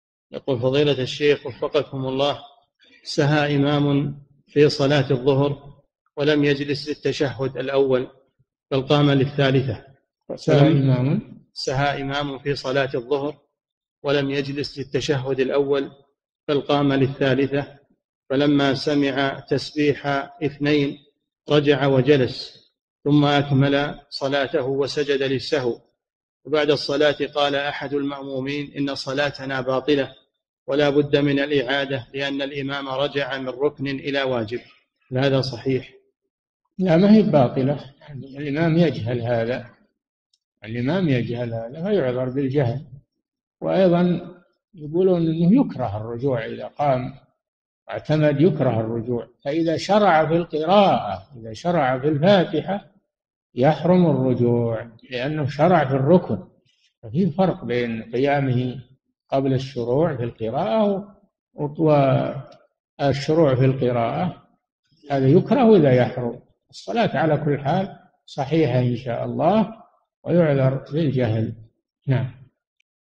[222 -521] إمام قام للثالثة ونسي التشهد الأوسط، ثم تذكر فرجع، فهل تبطل صلاته؟ - الشيخ صالح الفوزان
-[222 -521] السؤال: سها إمامٌ في صلاة الظهر ولم يجلس للتشهد الأول، بل قام للثالثة، فلما سمع تسبيح اثنين رجع وجلس، ثم أكمل صلاته وسجد للسهو، وبعد الصلاة قال أحد المأمومين: إن صلاتنا باطلة، ولابد من الإعادة؛ لأن الإمام رجع من ركنٍ إلى واجب، هل هذا صحيح؟
الجواب: لا ما هي بباطلة، الإمام يجهل هذا، فيُعذر بالجهل، وأيضًا يقولون: إنه يُكره الرجوع إذا قام واعتمد يُكره الرجوع، فإذا شرع في القراءة إذا شرع في الفاتحة يحرم الرجوع، لأنه شرع في الرُّكن، ففي فرق بين قيامه قبل الشروع في القراءة والشروع في القراءة هذا يُكره وهذا يحرم، الصَّلَاة على كل حال صحيحة إن شاء الله، ويُعذر بالجهل.
للاشتراك في قناة الشيخ صالح الفوزان

للاستفسار والإقتراحات والدعم
رئيس الفريق
لمشروع كبار العلماء